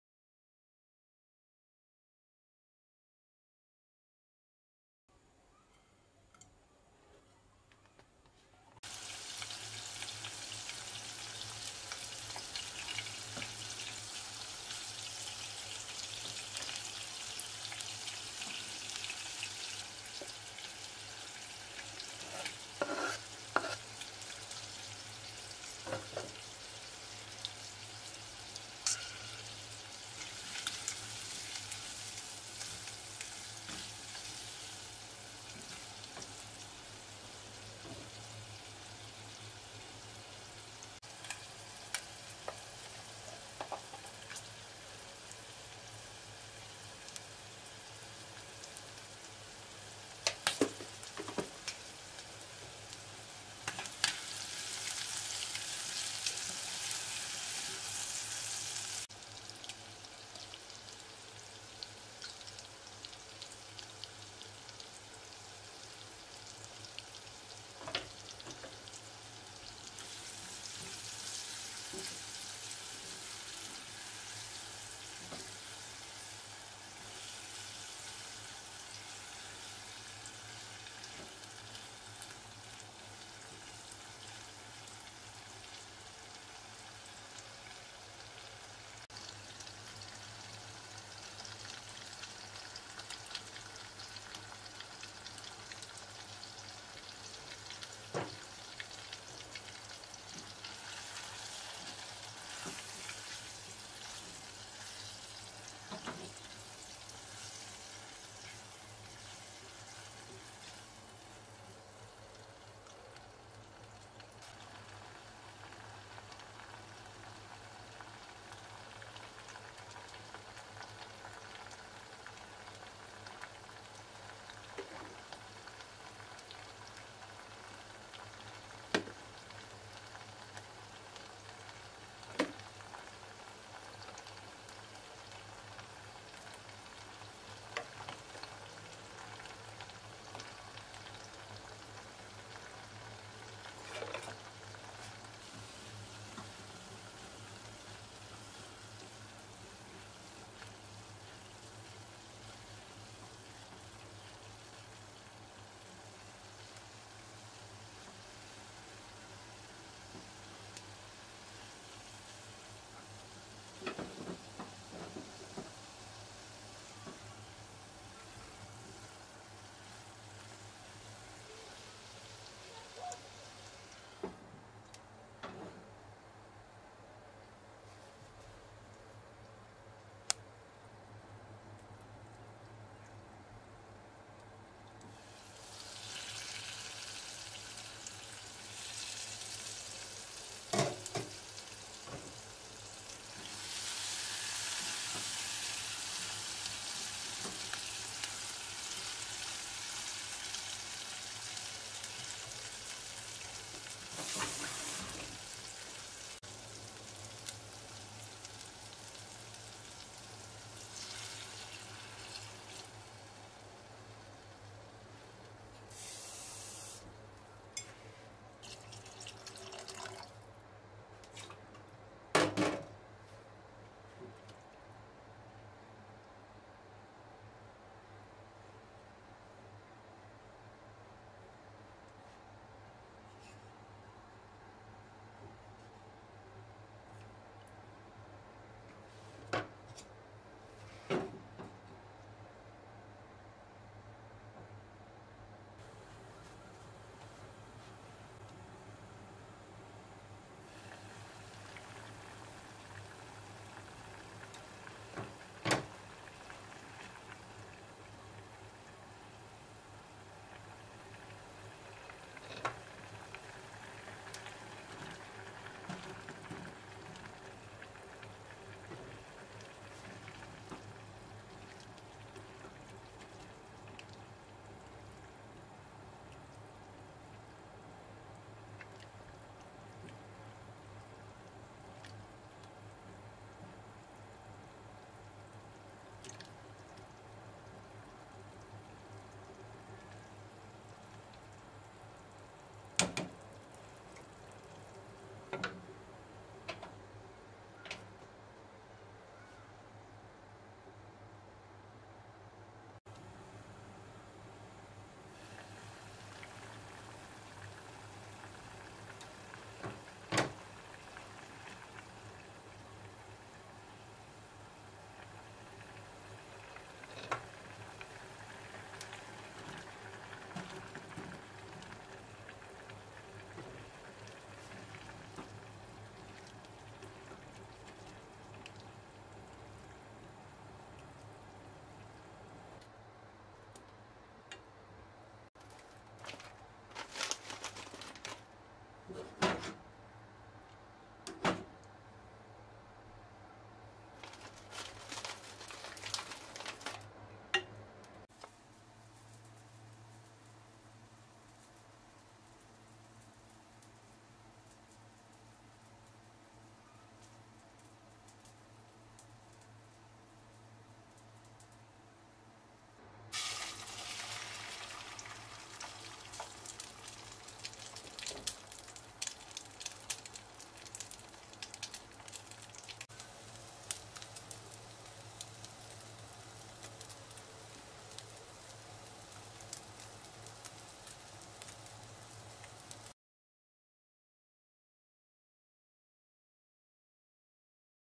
cauliflower curry / ডিম ভাজি সাদাভাত ফুলকপি আর চিংড়ি...
recipe video..

I love that cooking give me a chance to be creative and indulge in some 'play' time – something we adults could do with a lot more. I adore losing myself in the rituals of chopping, stirring and tasting.